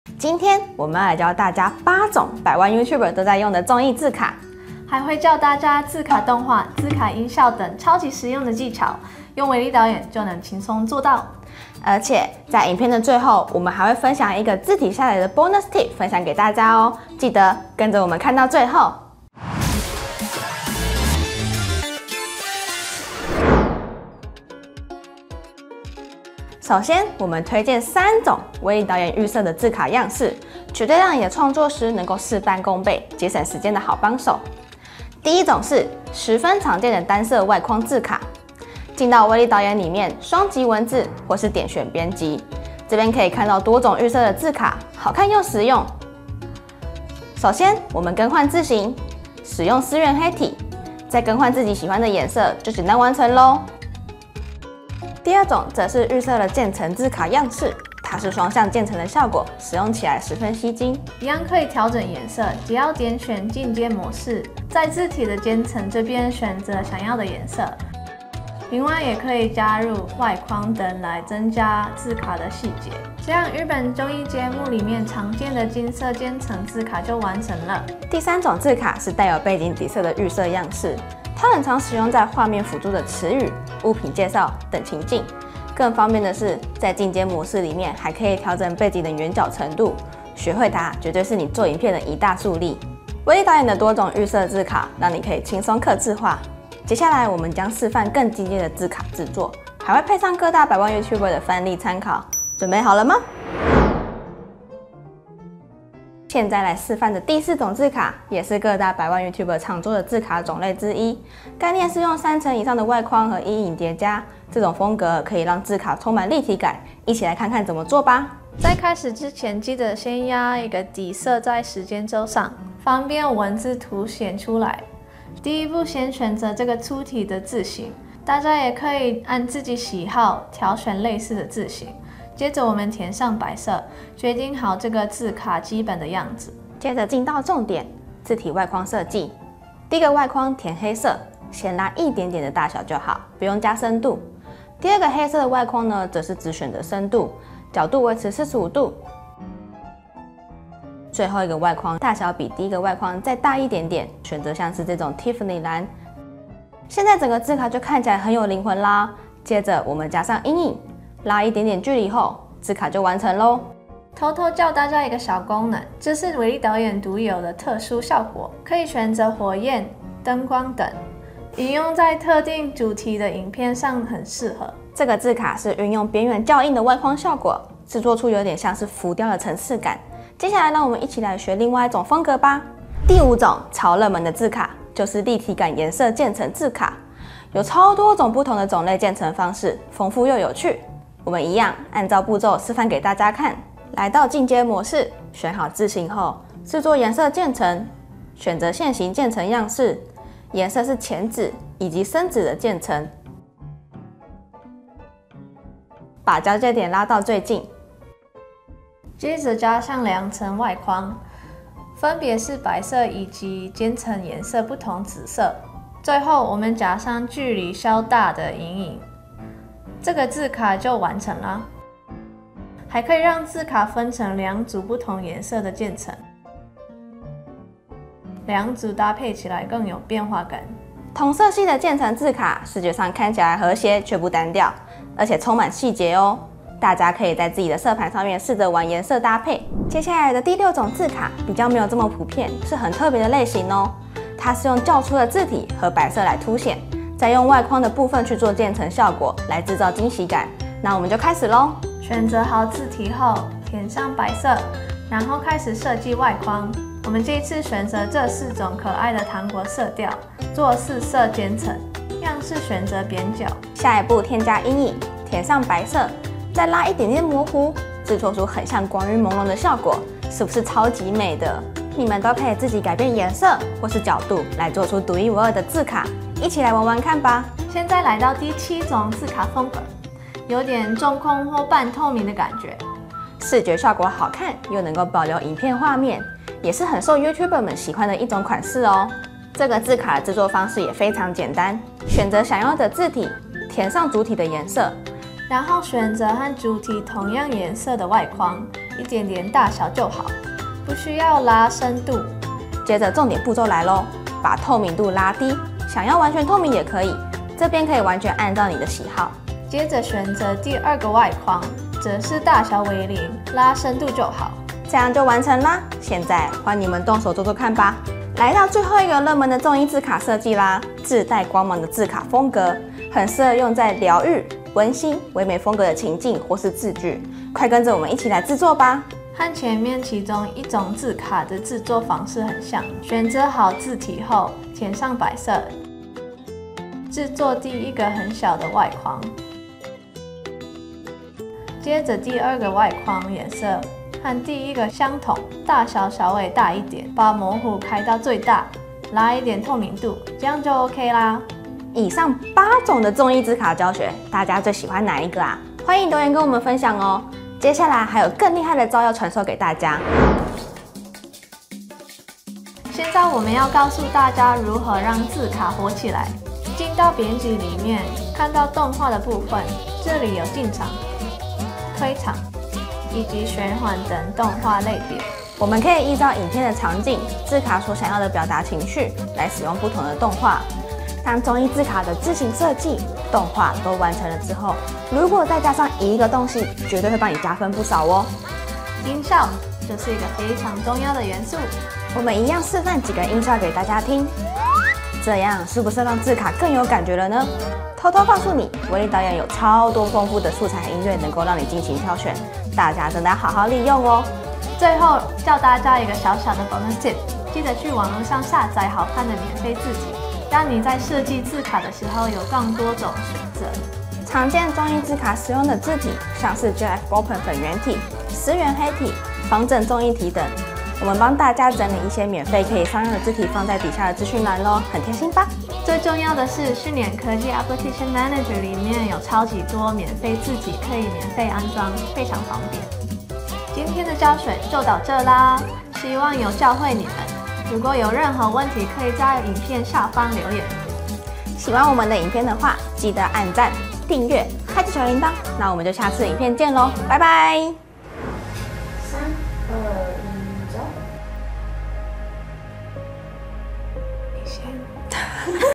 免費教你百萬YouTuber都在用的綜藝字卡製作方式｜威力導演電腦版教學
百萬YouTuber們都在用的 綜藝字卡 剪輯教學 來囉！🔥🔥🔥

想要讓你的影片字卡更生動、更有趣嗎？
只要看完這集！就可以了解綜藝字卡的製作邏輯，輕鬆掌握各種字卡風格！

今天威力導演官方教學頻道 免費教 大家八種綜藝字卡的製作方式，
這些字卡在威力導演裡面通通都可以完成！字卡製作全攻略一次分享給大家🙌

《Timecode》
00:00 開場介紹
00:23 三種簡單預設字卡教學
01:43 五種進階造型字卡教學
06:00 字卡動畫及音效
06:45 Bonus Tip - 網路開源字卡下載
07:13 Ending Talk
07:28 BTS

nov&utm_campaign=CyberLinkYouTubeChannel